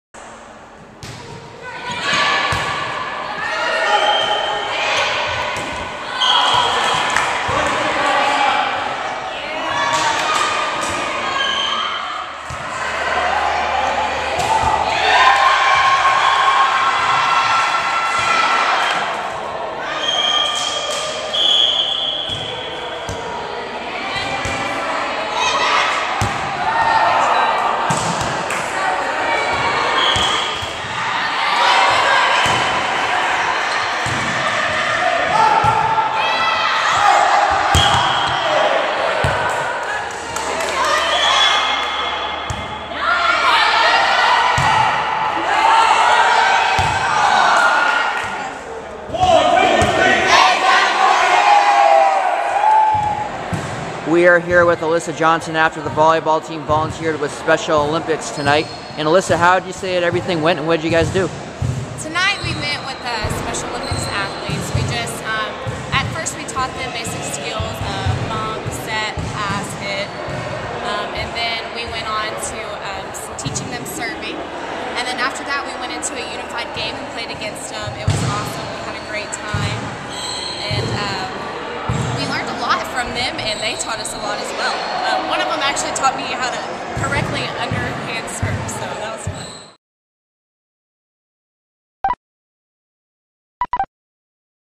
vball special olympics
The volleyball team volunteered with Special Olympics Texas on Tuesday evening. Alissa Johnson was interviewed afterwards about the experience.